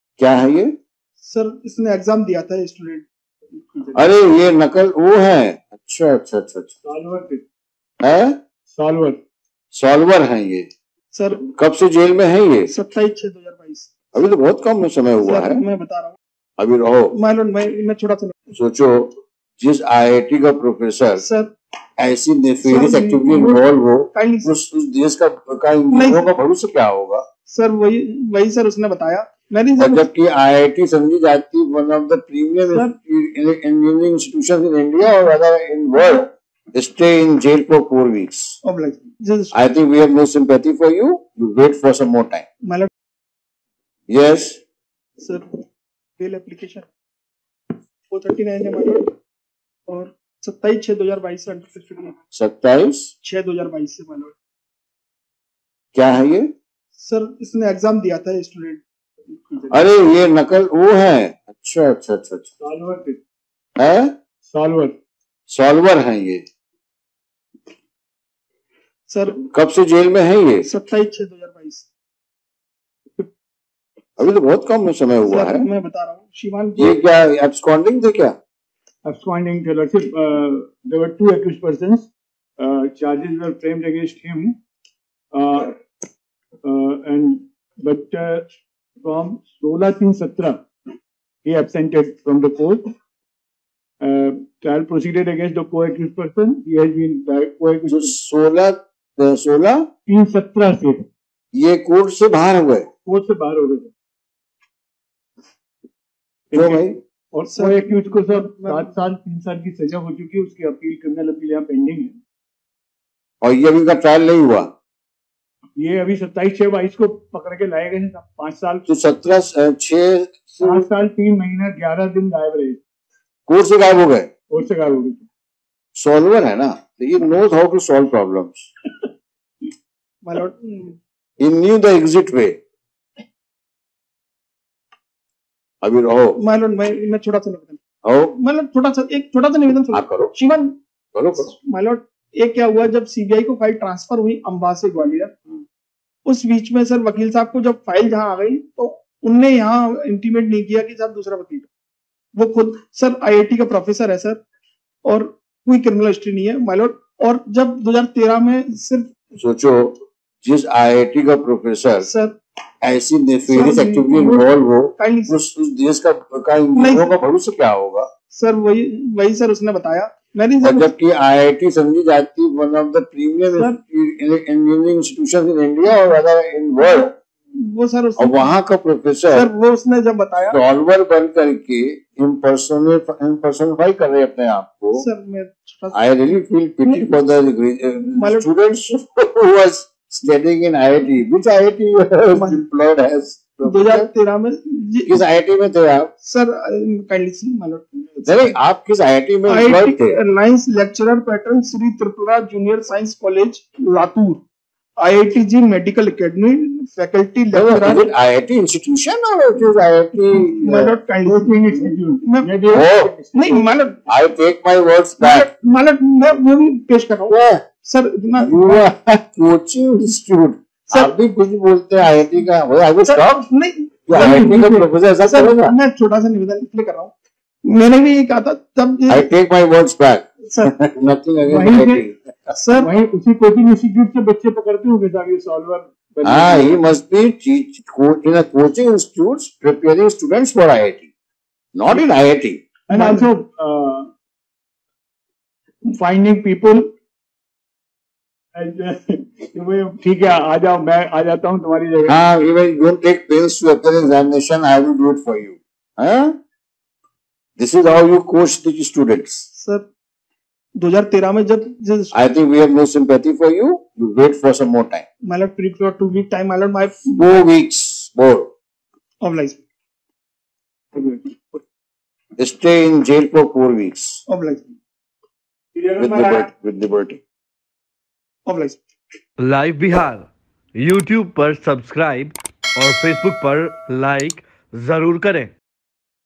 IIT EXAM में नकल कर रहा था छात्रप्रोफेसर निकला सॉल्वर गैंग का सदस्यजज ने सुना दिया फनी दलील
IIT EXAM में नकल कर रहा था छात्र प्रोफेसर निकला सॉल्वर गैंग का सदस्य जज ने सुना दिया फनी दलील